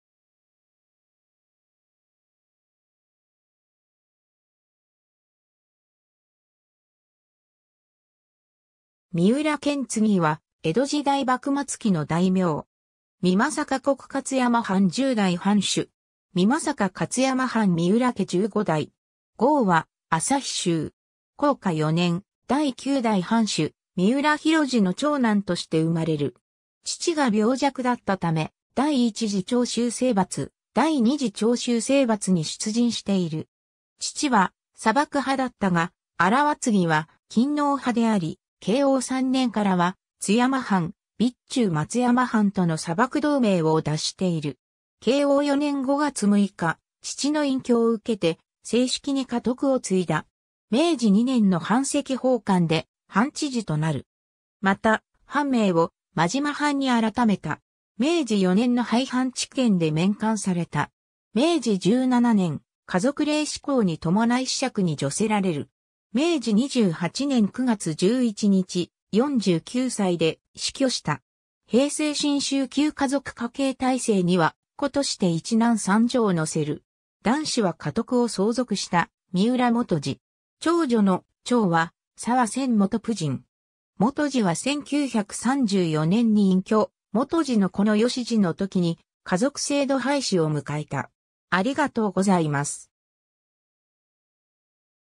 三浦顕次
三浦 顕次（みうら たかつぐ/あきつぐ）は、江戸時代幕末期の大名。
美作国勝山藩（真島藩）10代（最後の）藩主。
美作勝山藩三浦家15代。
号は旭州。
 弘化4年（1847年）、第9代藩主・三浦弘次の長男として生まれる。
 父が病弱だったため、第一次長州征伐・第二次長州征伐に出陣している。
父は佐幕派だったが、顕次は勤皇派であり、慶応3年（1867年）からは津山藩（松平慶倫）・備中松山藩（板倉勝静）との佐幕同盟を脱している。
慶応4年（1868年）5月6日、父の隠居を受けて正式に家督を継いだ。
明治2年（1869年）の版籍奉還で藩知事となる。
また、藩名を真島藩に改めた。
明治4年（1871年）の廃藩置県で免官された。
 明治17年（1884年）、華族令施行に伴い子爵に叙せられる。
明治28年（1895年）9月11日、49歳で死去した。
 『平成新修旧華族家系大成』には、子として1男3女を載せる。
男子は家督を相続した三浦基次（1872年 - 1946年）。
長女の「ちょう」は澤宣元（男爵）夫人。
 基次は1934年に隠居、基次の子の義次（1904年 - 1985年）のときに華族制度廃止を迎えた。